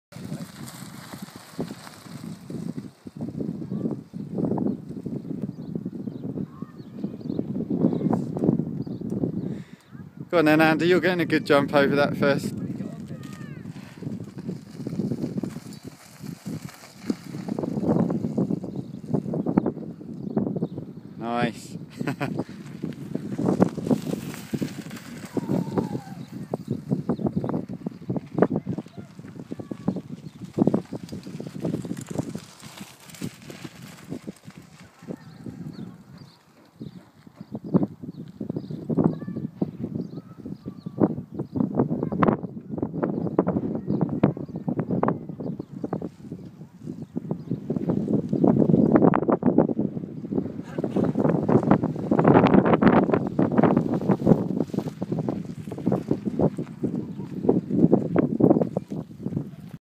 Poole BMX Track - old school bmx ride out Easter 2013
Me and some local friends on an old school bmx ride out, taking in the 1980s BMX track at Poole, Dorset, UK. The track is now a little overgrown and very sandy in places but still rideable and good fun.